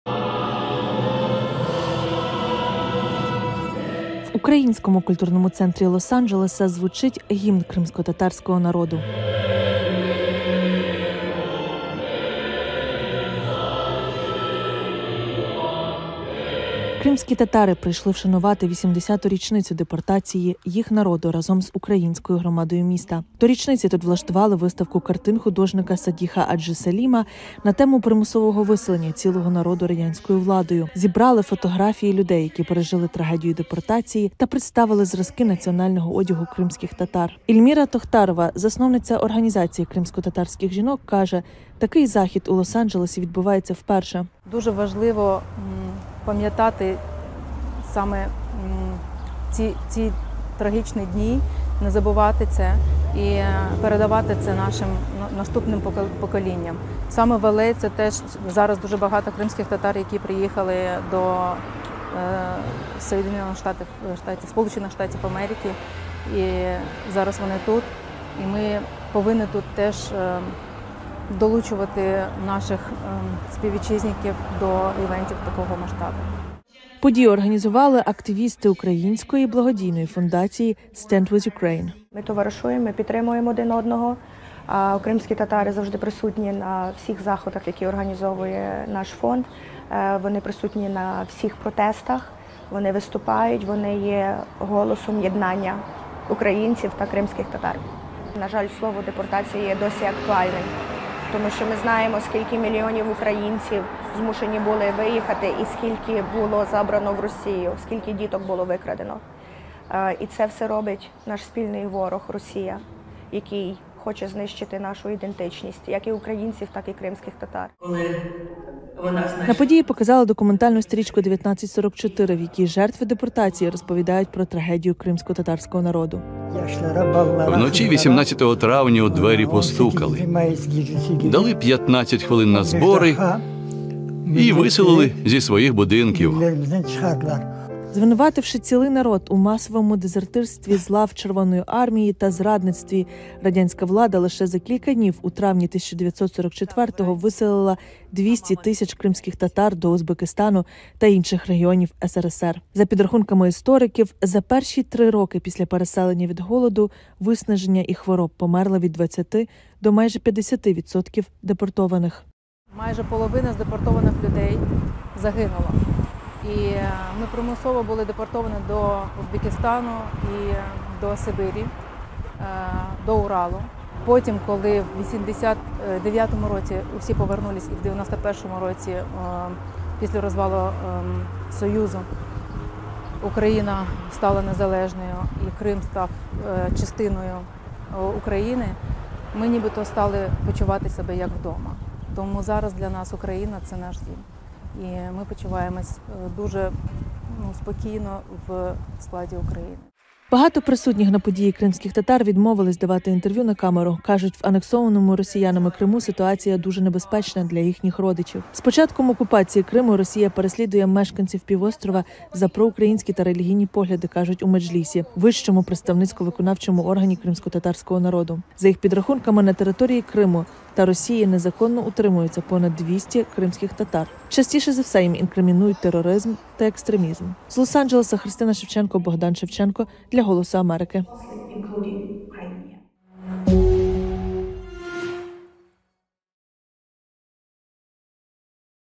Crimean Tatar Culture Day in LA: Heritage, Resilience, and Remembrance held by SWUF
Crimean Tatar Culture Day: Heritage, Resilience, and Remembrance.
80th Anniversary of the Genocide of Crimean Tatars organized by  Stand With Ukraine Foundation and Crimean Tatar Women Initiative
День Кримськотатарської Культури: спадщина, стійкість і пам'ять.
80-ті роковини геноциду кримських татар. Захід організовано Stand With Ukraine Foundation та Ініціативи Кримсько-Татарських Жінок.
Репортаж Христина Шевченко, Голос Америки
Травень, 2024